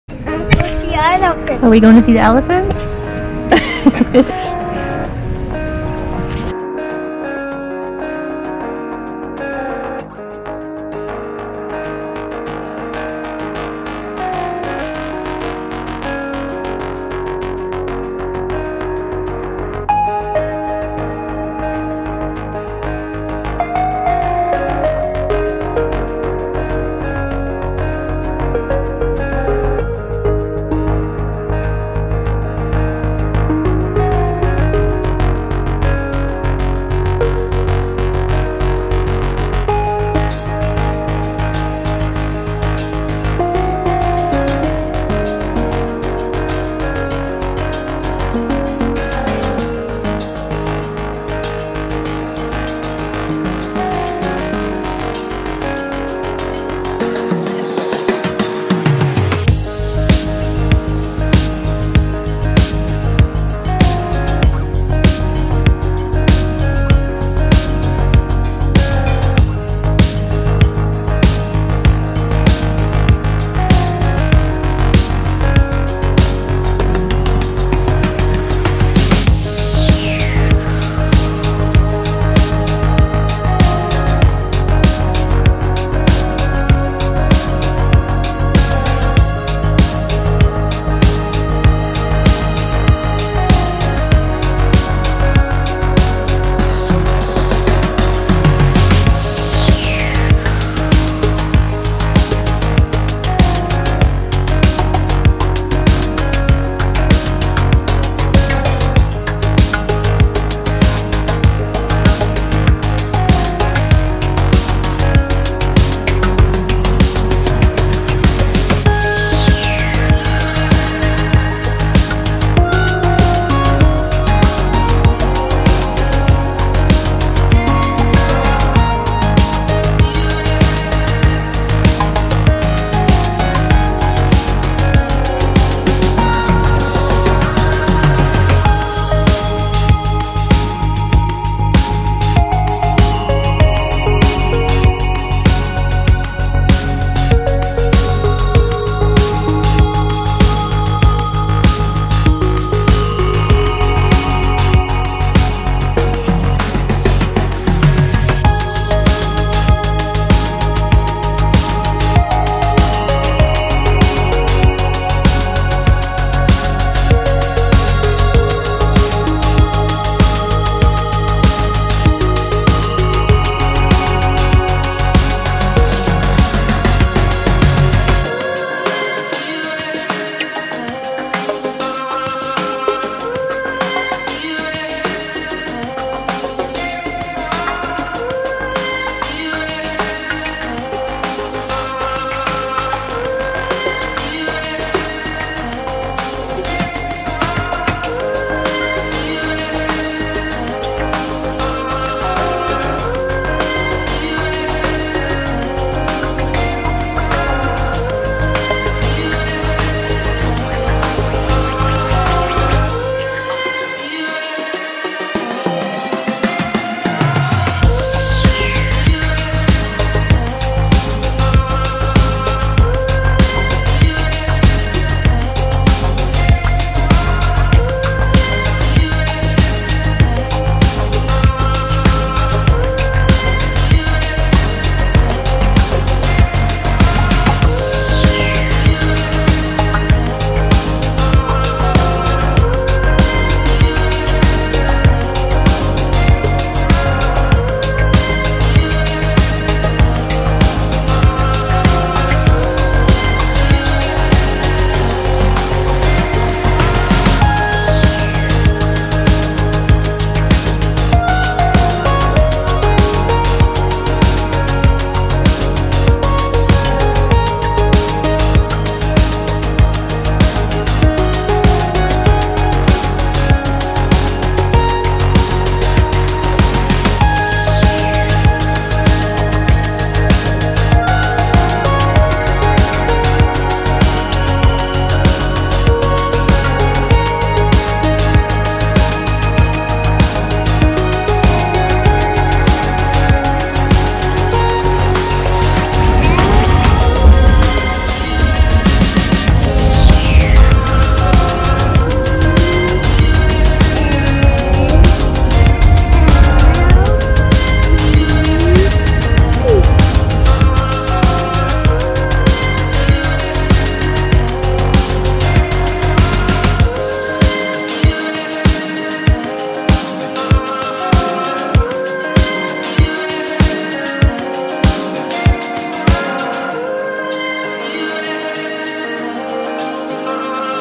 Toledo Zoo // July 2019 // TOLEDO, OHIO
Trip to the Toledo Zoo with my nephews, sister & bro-in-law.
July 2019

What I film with:
GoPro Hero 7
GoPro Hero 6
Selfie Stick/Tripod
Memory Card